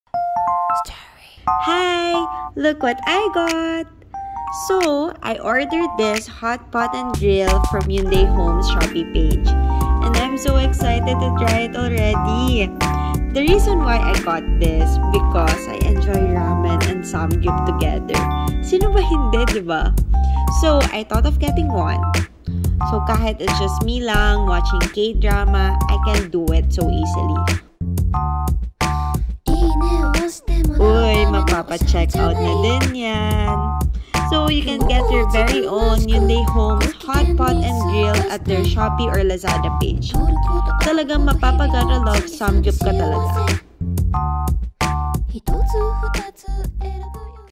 A must-have Hotpot Grill for your Korean cravings!
Craving some Hotpot and Korean Barbeque right now. 👀

What about you, chingus? Heat up and grill your fave Korean dish at home with the Hyundai Home hotpot and grill today!

Just in time for the cold season. ♥️♥️♥️♥️